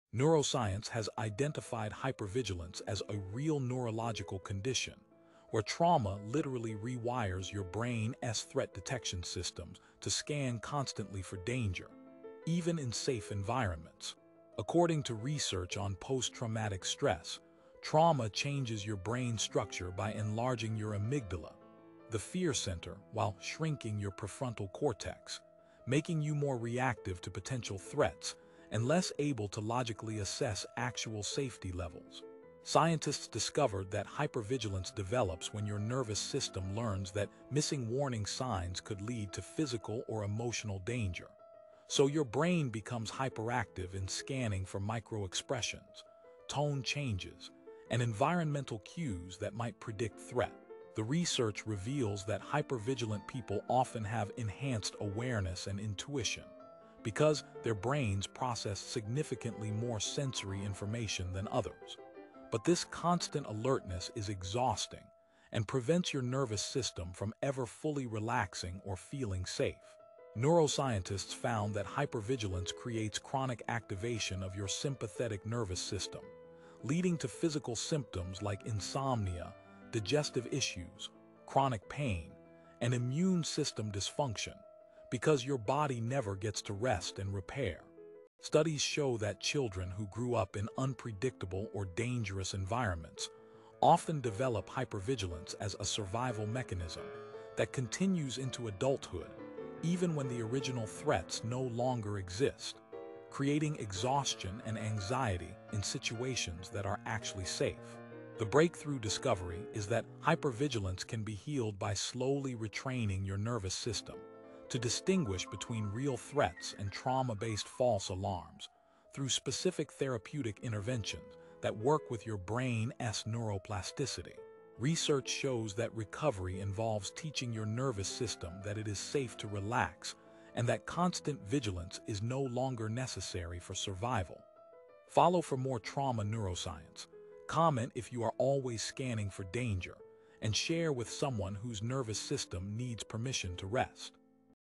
Why Trauma Makes You Hypervigilant
Neuroscience explains how trauma rewires your threat detection systems and creates exhausting constant alertness to potential danger.